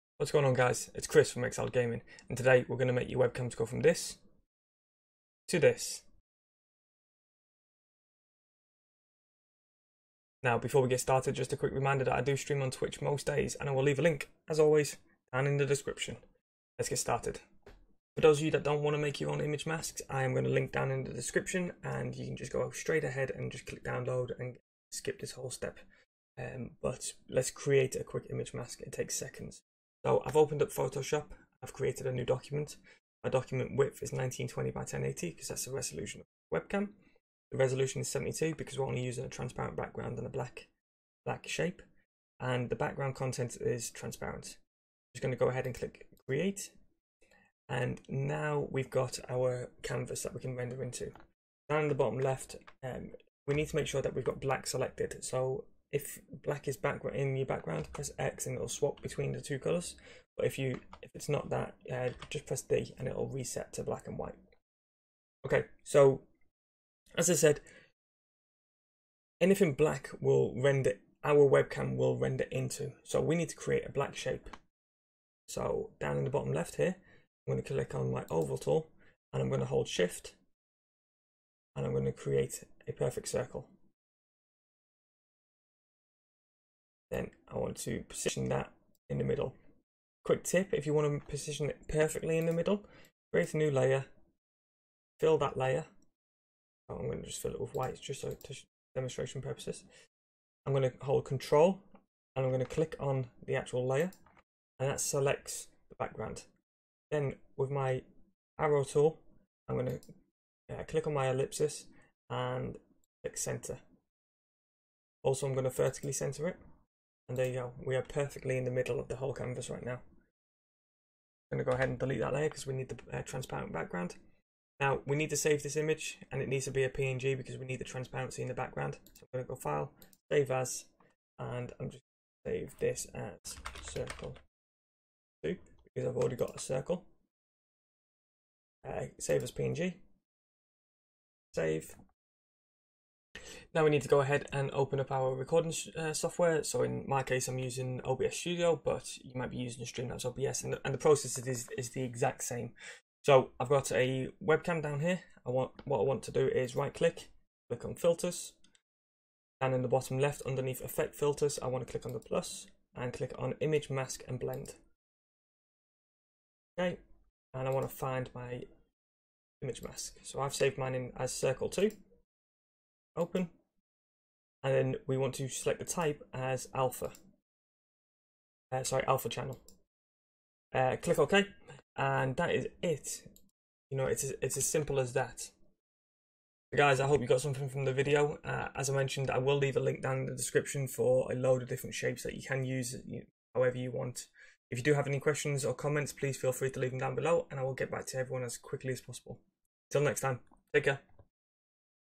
How to use custom webcam shapes! - OBS Image/Mask Blend Tutorial
Today I walk you through step by step on how to get started with live streaming through OBS Studio (it's easier than you think)

⏩ Timestamps, click to skip ahead!
0. Introduction: 0:00
1. Make image masks: 0:17
2. Add image mask to webcam: 2:23
3. Outro: 3:06